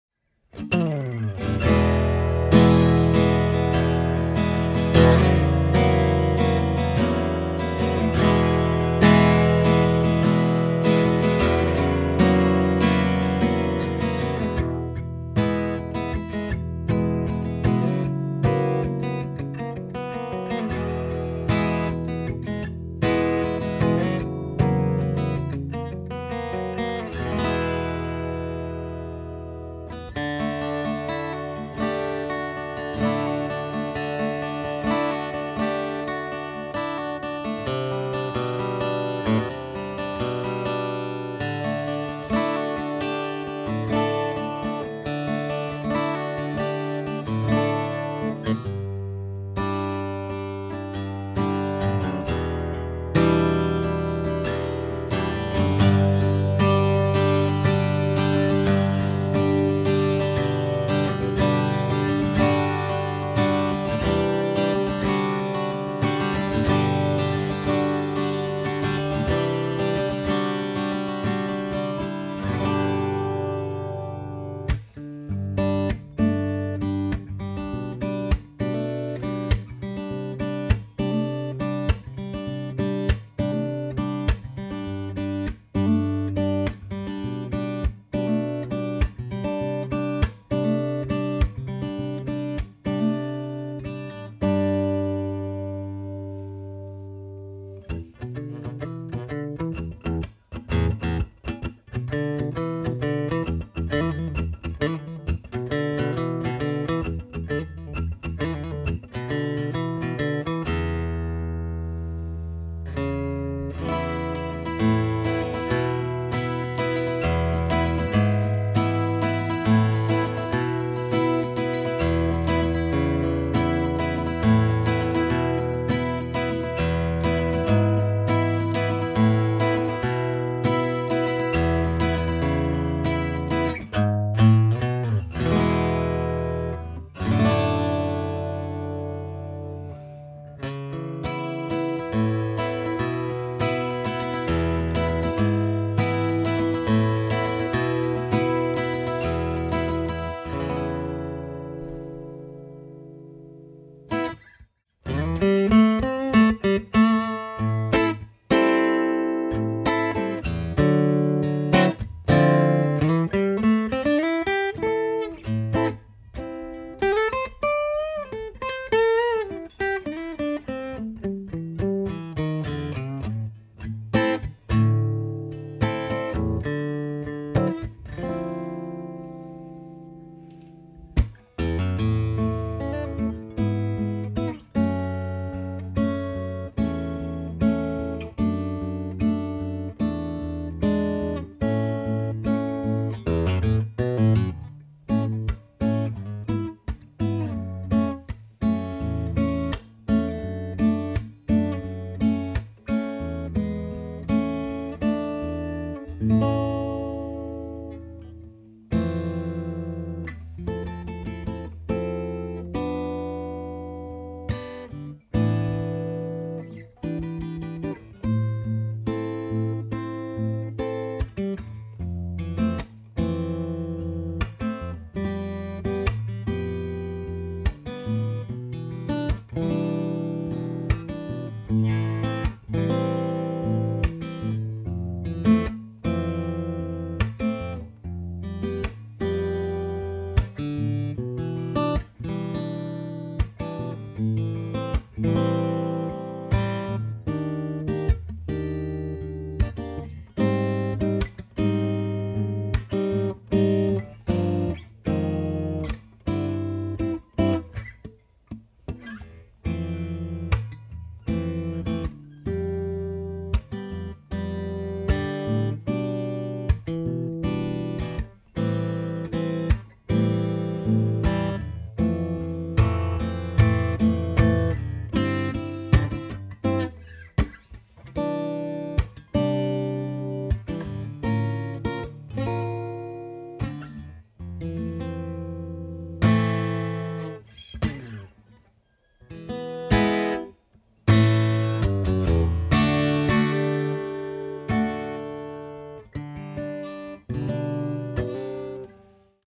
Teste violão Byron T-10 (Taylor Killer?)
Nesse vídeo faço uma rápida demonstração de um violão chinês feito sob encomend, o Byron T-10, que nada mais é do que um modelo inspirado no Taylor 914ce. O objetivo não é dizer se é melhor ou pior do que um Taylor, até porque os instrumentos estão em faixas de preço completamente distintas. O intuito aqui é apenas mostrar que se pode ter bons instrumentos pagando menos do que custam bons violões das grandes marcas. E é claro que não apoio qualquer tipo de falsificação de instrumentos.